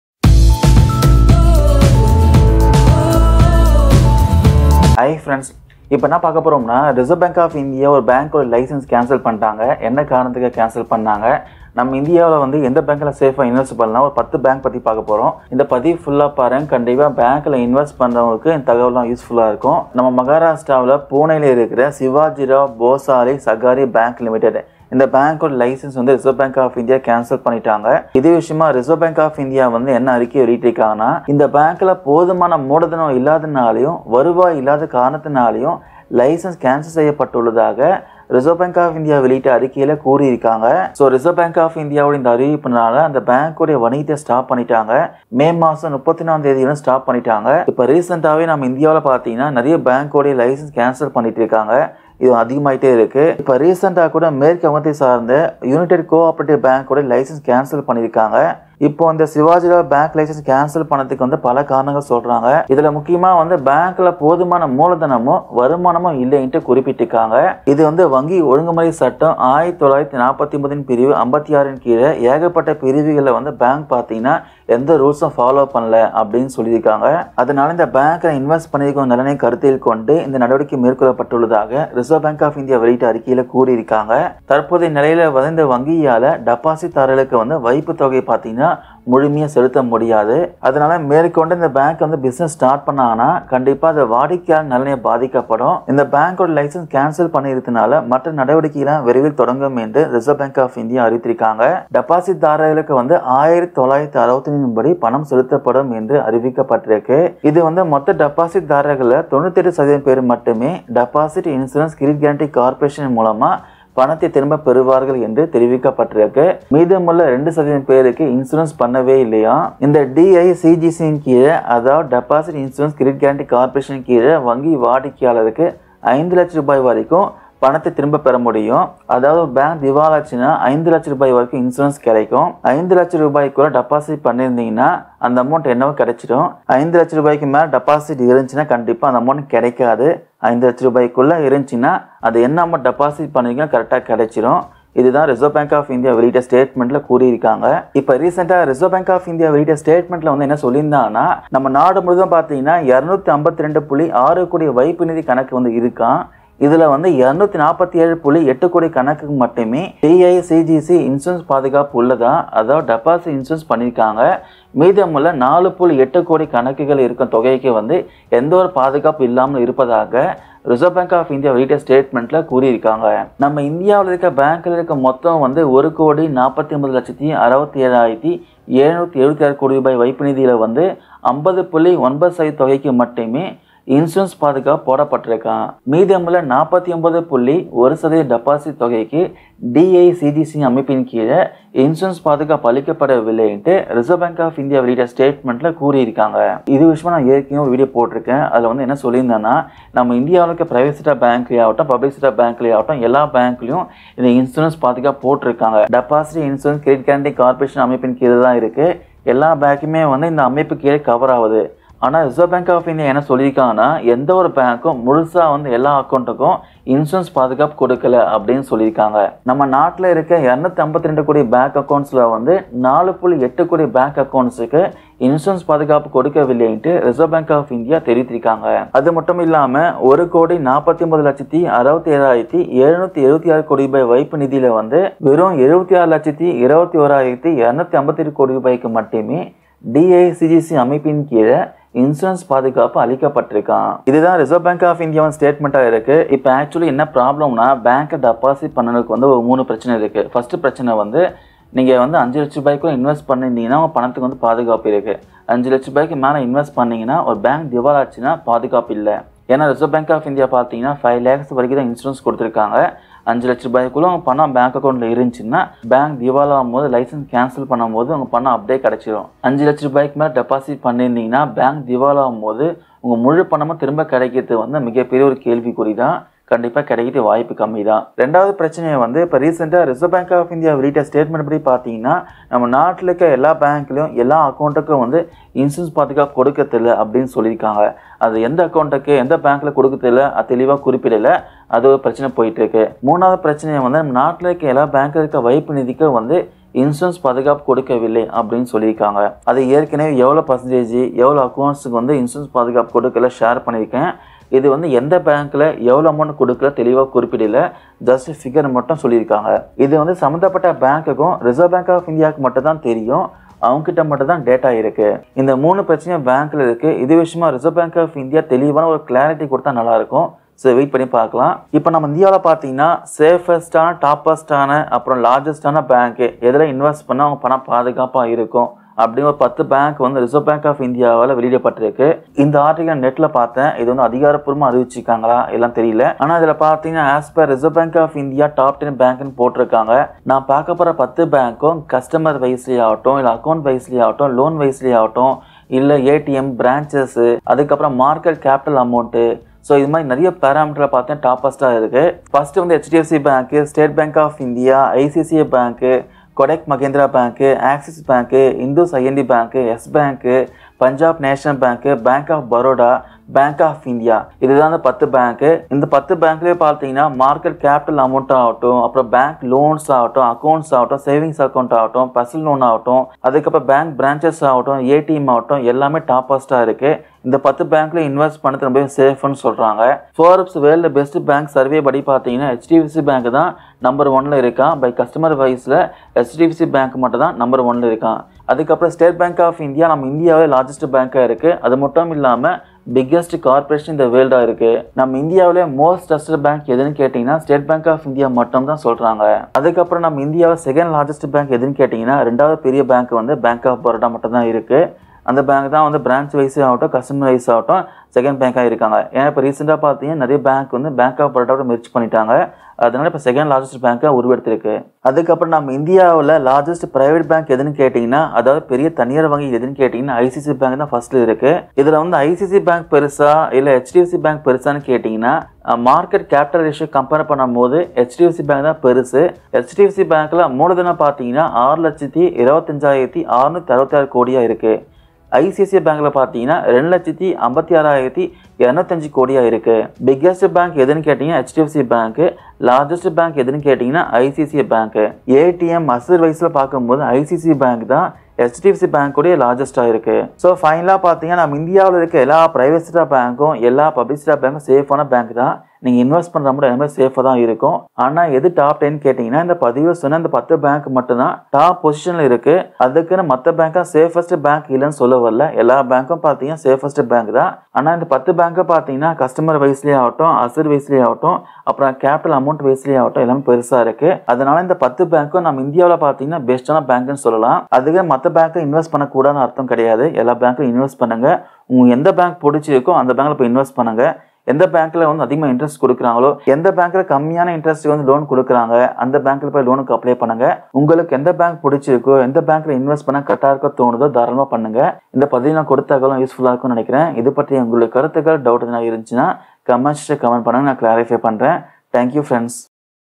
RBI எடுத்த அதிரடி முடிவு🔥இந்த 10 வங்கிகளில் பணம் போடாதே🔥அடுத்தடுத்து திவாலாகும் வங்கிகளின் பட்டியல்🔥
🔥RBI எடுத்த அதிரடி முடிவு!🔥 இந்த 10 வங்கிகளில் பணம் போடாதே!🔥 அடுத்தடுத்து திவாலாகும் வங்கிகளின் பட்டியல் ரெடி🔥 பாதுகாப்பான 10 வங்கி பட்டியல் வெளியீடு! மக்களே உஷார்🔥

🔥உங்க வங்கி டெபாசிட் பாதுகாப்பா இருக்கா..? குண்டை போட்ட ரிசர்வ் வங்கி.. மக்கள் அதிர்ச்சி..!

மகாராஷ்டிராவில் உள்ள புனேவில் சிவாஜிராவ் போசாலே சஹாகரி வங்கி லிமிட்டெட் நிறுவனத்தின், வங்கி உரிமத்தினை ரிசர்வ் வங்கி ரத்து செய்துள்ளது.

இது குறித்து ரிசர்வ் வங்கி வெளியிட்டுள்ள அறிக்கையில், இவ்வங்கியில் போதுமான மூலதனம் இல்லாததாலும், வருவாய் இல்லாமையாலும் உரிமத்தினை ரத்து செய்துள்ளதாக தெரிவித்துள்ளது.

கிரீன் கார்டு கட்டுப்பாட்டில் மிகப்பெரிய தளர்வு.. இந்தியர்களுக்கு அடித்தது யோகம்.. இனி ரொம்ப ஈசி..!

மே 28,2021 தேதியிட்ட உத்தரவின் படி, மகாராஷ்டிராவின் புனேவை சேர்ந்த சிவாஜிராவ் போசாலே சகாரி வங்கி லிமிடெட் நிறுவனத்தின் உரிமத்தினை ரத்து செய்துள்ளது.

வங்கி உரிமம் ரத்து
ரிசர்வ் வங்கியின் இந்த அறிவிப்பினால் வங்கி வணிகத்தினை மேற்கொள்வதை நிறுத்திக் கொண்டுள்ளதாகவும், இதே மே 31, 2021வுடன் வணிகத்தினை மூடியுள்ளதாகவும் தெரிவித்துள்ளது. இந்தியாவில் இது போன்ற அடுத்தடுத்த வங்கிகளின் உரிமம் ரத்து செய்யப்படுவது அதிகரித்து வருகின்றது. சமீபத்தில் மேற்கு வங்கத்தைச் சேர்ந்த யுனைடெட் கோ ஆப்ரேட்டிவ் வங்கி உரிமத்தை ரிசர்வ் வங்கி ரத்து செய்தது நினைவு கூறத்தக்கது.

என்ன காரணம் ?
மகாராஷ்டிராவைக் தளமாகக் கொண்ட இந்த வங்கியின் உரிமத்தினை ரத்து செய்தற்காக, பல காரணங்களை மேற்கோளிட்டுள்ளது ரிசர்வ் வங்கி. குறிப்பாக வங்கி போதிய மூலதனம் மற்றும் வருமானம் இல்லை. இது வங்கி ஒழுங்குமுறை சட்டம், 1949ன் பிரிவு 56 உடன் படித்த பிரிவு 11(1) மற்றும் பிரிவு 22(3) விதிகளுக்கு இணங்கவில்லை என்றும் தெரிவித்துள்ளது.

இந்த விதிகளை பின்பற்றவில்லை
இது தவிர பிரிவு 22(3)(3) (a), 22(3)(b), 22(3)(c), 22(3)(d) மற்றும் 22(3)(e) உள்ளிட்ட பிரிவுகளின் கீழ் வங்கியானது இணங்க தவறி விட்டது. அதோடு 1956ன் பிரிவு 56ன் கீழ், ரிசர்வ் வங்கி, ஏற்கனவே வழிகாட்டுதல் உத்தரவினை பிறப்பித்திருந்தது. இதனால் ரிசர்வ் வங்கியின் வழிகாட்டுதல்களின் கீழ் இவ்வங்கியானது இருந்தது.

மக்கள் நலன் முக்கியம்
மேலும் இவ்வங்கியின் டெபாசிட்டர்களின் நலனைக் கருத்தில் கொண்டும் இந்த நடவடிக்கையின ரிசர்வ் வங்கி எடுத்துள்ளதாக தெரிவித்துள்ளது. தற்போதைய நிலையில் இவ்வங்கியால் டெபாசிட்தாரர்களுக்கு வைப்பு தொகையை முழுமையாக செலுத்த முடியாது. ஆக இவ்வங்கி, மேற்கொண்டு வங்கி தொழிலை தொடர்ந்தால், வாடிக்கையாளர்களின் நலன் பாதிக்கப்படும்.

பணம் என்ன ஆகும்?
இவ்வங்கியின் உரிமம் ரத்தும் செய்யப்பட்டுள்ள நிலையில், மற்ற நடவடிக்கைகளை விரைவில் தொடங்கும் என அறிவித்துள்ளது. டெபாசிட்தாரர்களுக்கு 1961ன் படி பணம் செலுத்தப்படும் எனவும் அறிவித்துள்ளது. இது மொத்த டெபாசிட்தார்களில் 98% பேர் Deposit Insurance and Credit Guarantee Corporation மூலம் தொகையை திரும்ப பெறுவார்கள் எனவும் தெரிவித்துள்ளது.

ரூ.5 லட்சம் வரை பெறலாம்
ஆக DICGC கீழ் வங்கி வாடிக்கையாளர்களுக்கு 5 லட்சம் ரூபாய் வரையிலான டெபாசிட்டினை திரும்ப பெற்றுக் கொள்ளலாம். இதனால் மக்களின் டெபாசிட் பணம் முழுமையாகக் கிடைக்கும். யாரும் பயப்பட தேவையில்லை எனவும் அறிவிக்கப்பட்டுள்ளது. எனினும் இதில் 5 லட்சம் ரூபாய் வரையில் தான் இந்த திட்டத்தில் திரும்ப பெற முடியும்.